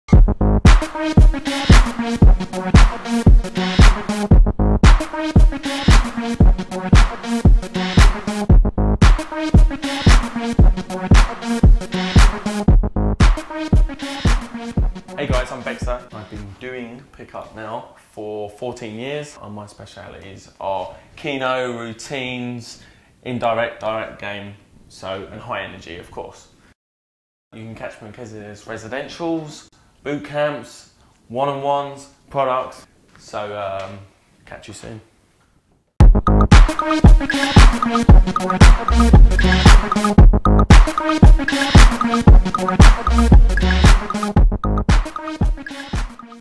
Pick Up Artist "BECKSTER"
Although Kezia and her team of expert PICK UP coaches and wing girls tend to stay away from 'routine based' techniques, there is still a demand to learn fun and easy ways that will help a guy increase his success rate with women, Beckster is one of the most well known 'routine' based PICK UP COACHES in europe. For more information about Beckster and the rest of the team, go